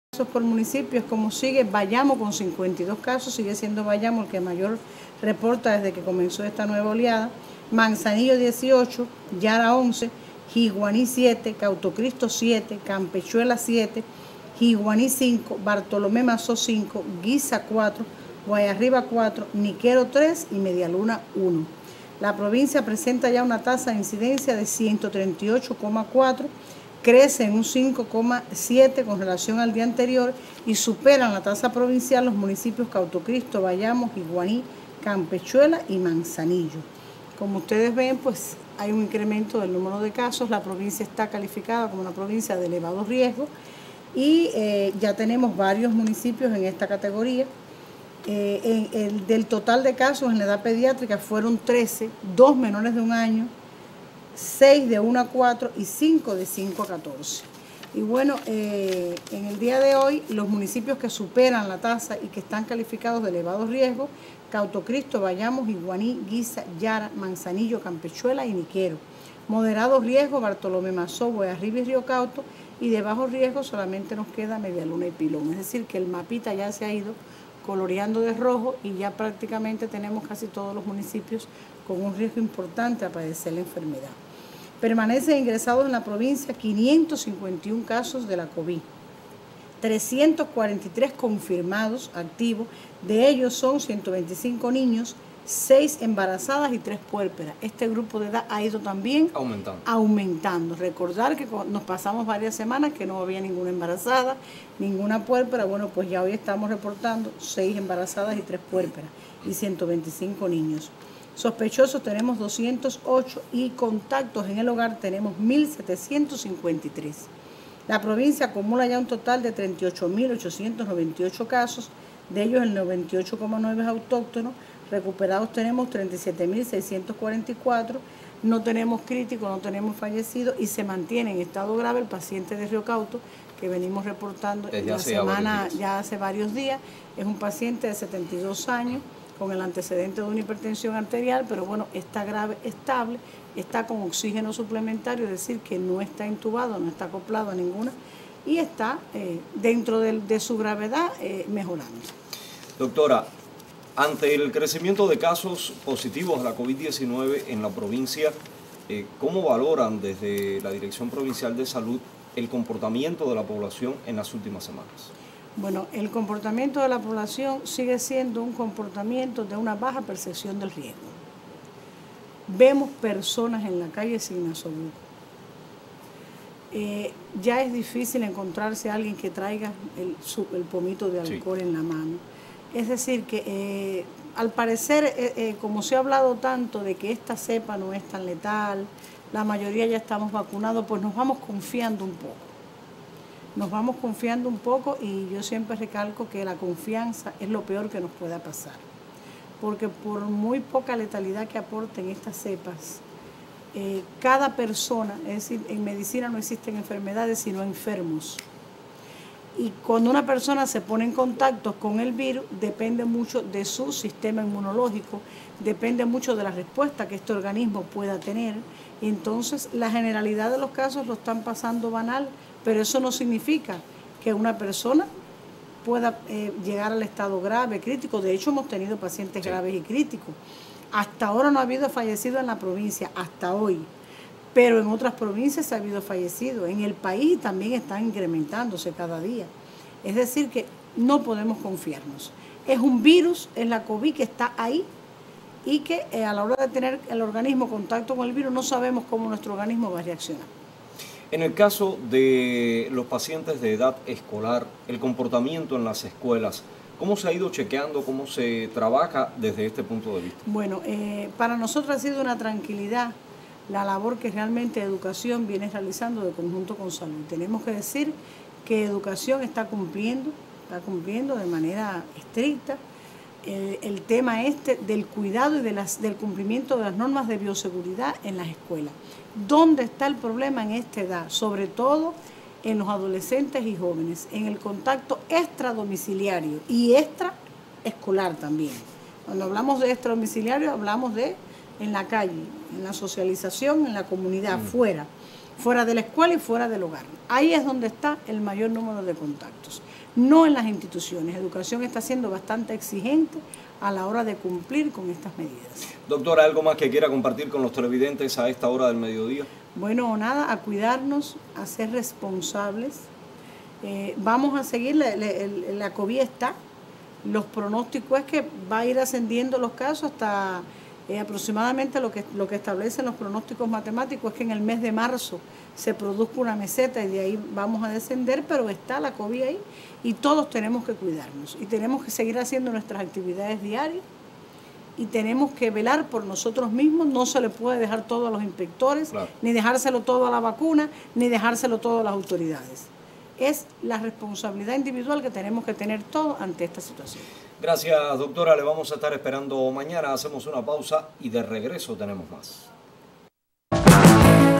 Se complejiza un poco más en Granma situación con la Covid-19
La Doctora Kenia González Medina, vicedirectora de Atención médica en la Dirección provincial de Salud, al actualizar las cifras, como cada día en la revista televisiva Frecuencia 12,  definió el comportamiento de la población de “baja percepción del riesgo”, al encontrar en la calle a personas sin nasobuco, o usándolo incorrectamente; y muchas de ellas sin el pomo para la desinfección de las manos.